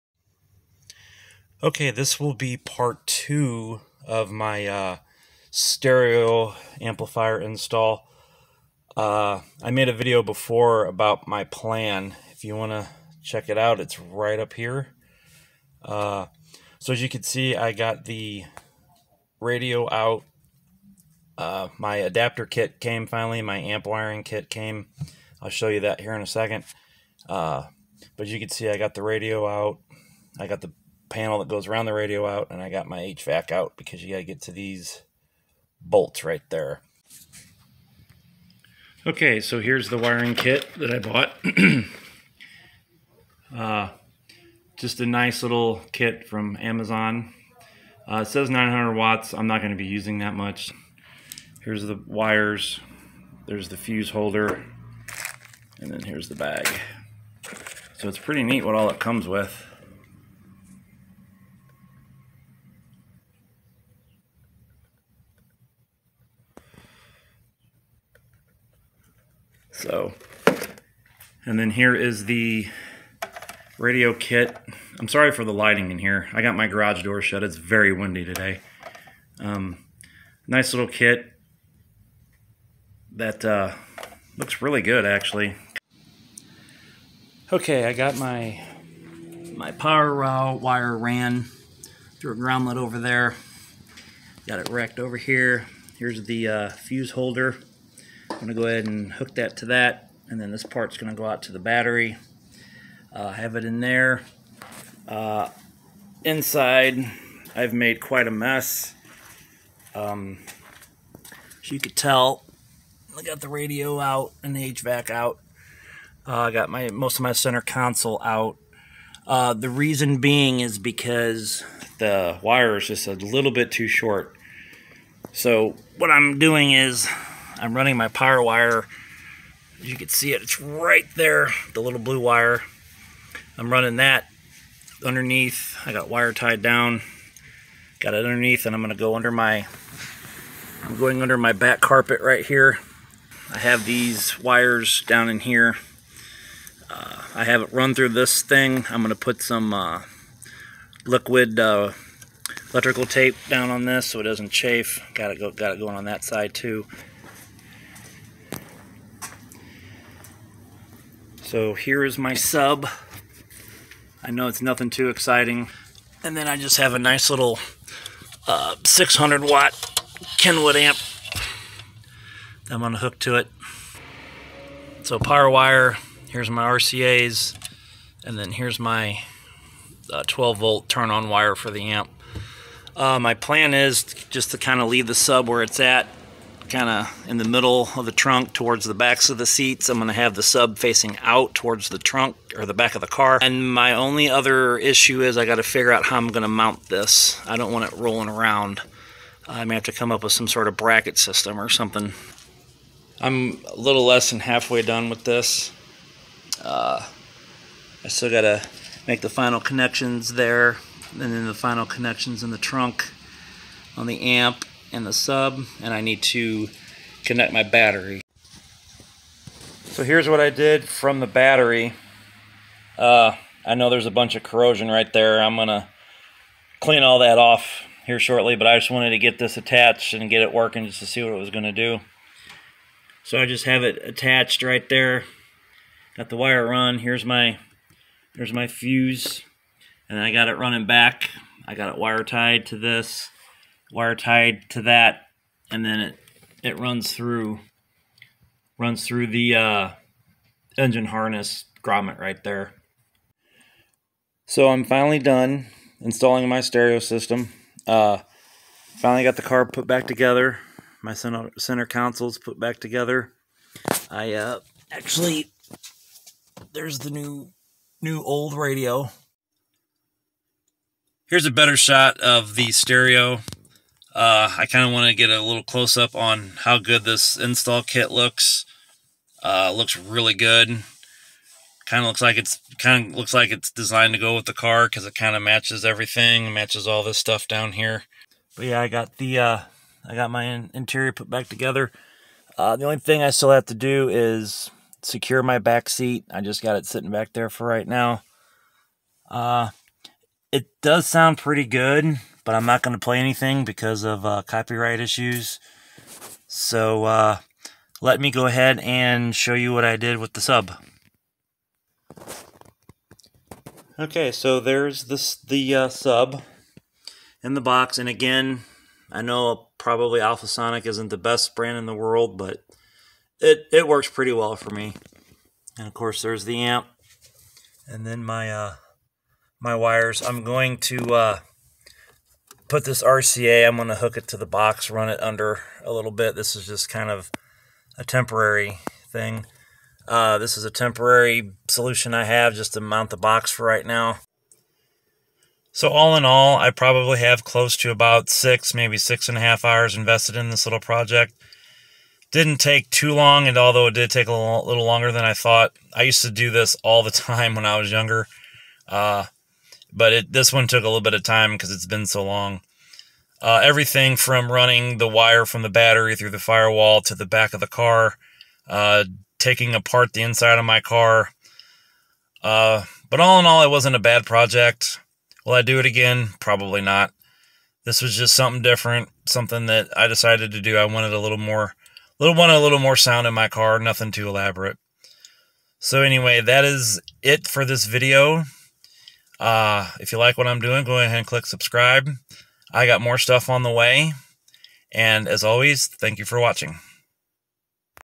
Part 2 of changing out my factory car stereo
Part 2 and final install of a newer stereo, amp and sub in my 2004 Camry. I removed the factory stereo and decided to replace it with a newer one with a small amp and a subwoofer. I show some of the install in this video.